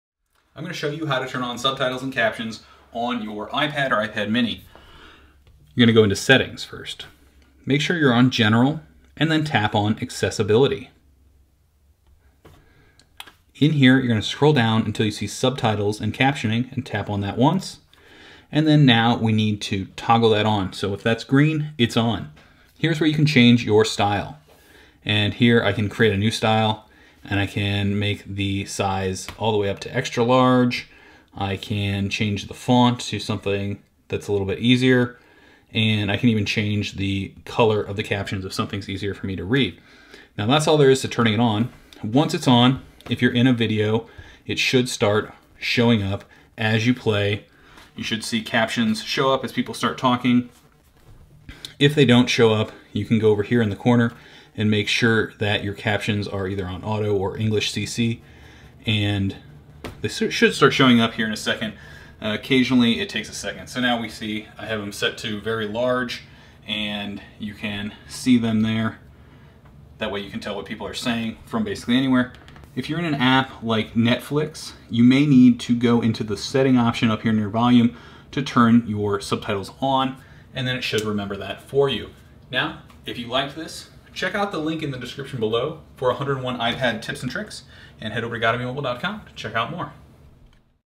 How to Use Captions & Subtitles iPad
Turn on iPad Captions and Subtitles to make sure you can see what people are saying on the iPad. More